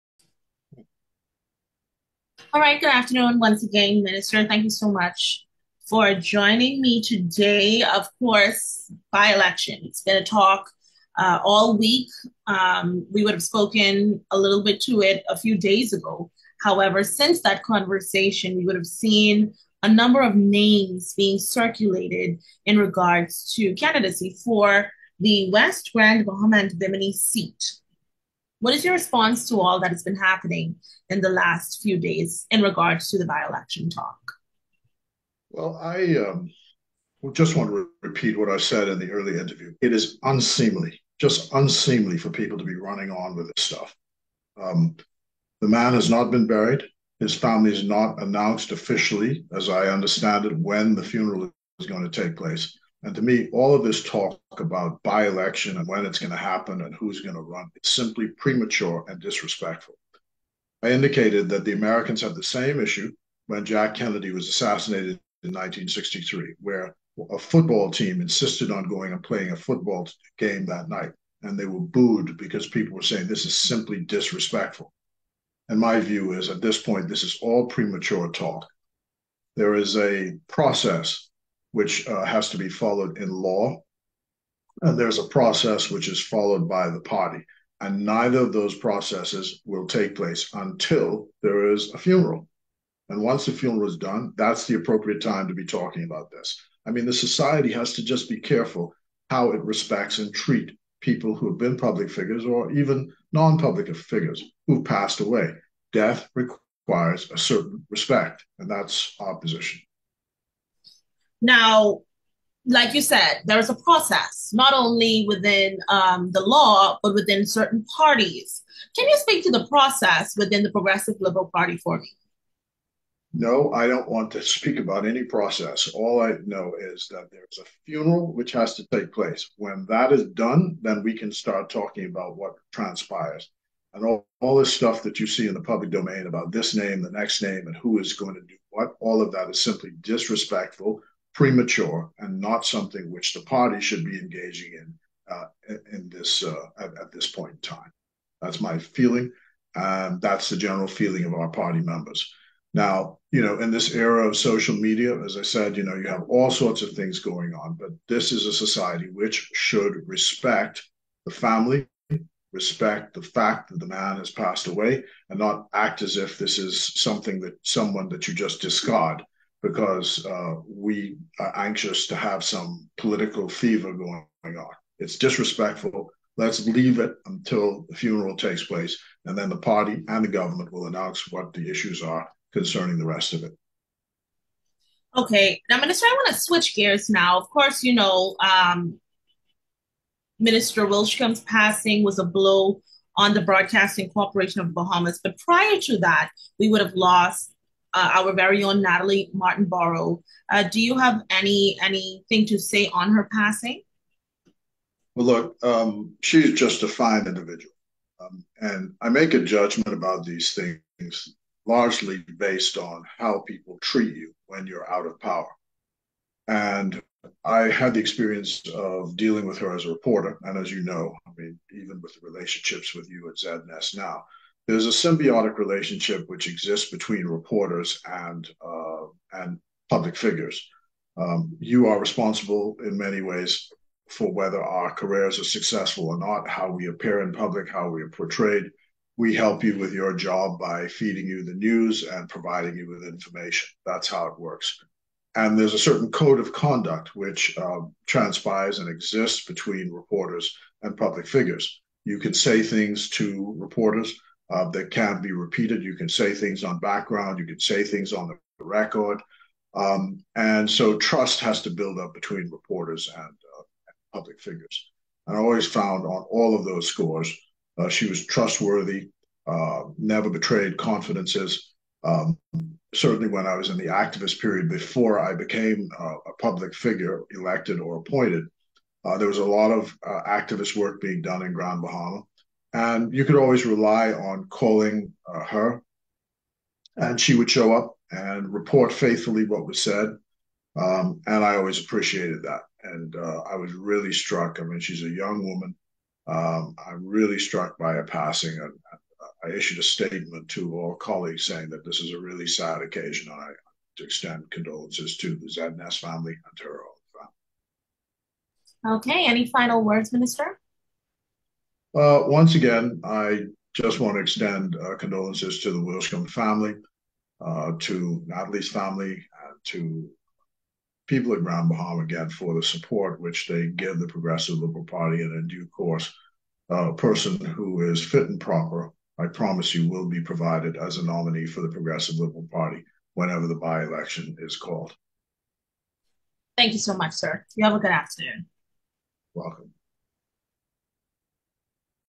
Fred Mitchell speaks to respect for the passing of Obie Wilchcombe with Azure Quant of ZNS North.
Fred Mitchell, Chair of the PLP, speaks to respect for the passing  of Obie Wilchcombe with Azure Quant of ZNS Northern Service. 28 September 2023

Statement From The Chairman Progressive Liberal Party
On The Passing of The Hon Obie Wilchcombe
For Immediate Release
28 September 2023

Our nation has lost a giant in the person of the Hon. Obie Wilchcombe, Minister of Social Services, Information and Broadcasting and Member of Parliament for West Grand Bahama and Bimini.

In short order, the Government of The Bahamas is expected to make a formal announcement on official funeral arrangements.

As the country is in mourning, it is inappropriate for the Progressive Liberal Party to be accepting any requests or expressions of interest for candidacy before the funeral or any formal processes in parliamentary law have begun.

Minister Wilchombe has served our nation and party well. We remain focused on honoring his contributions and legacy.

The public is so advised.

End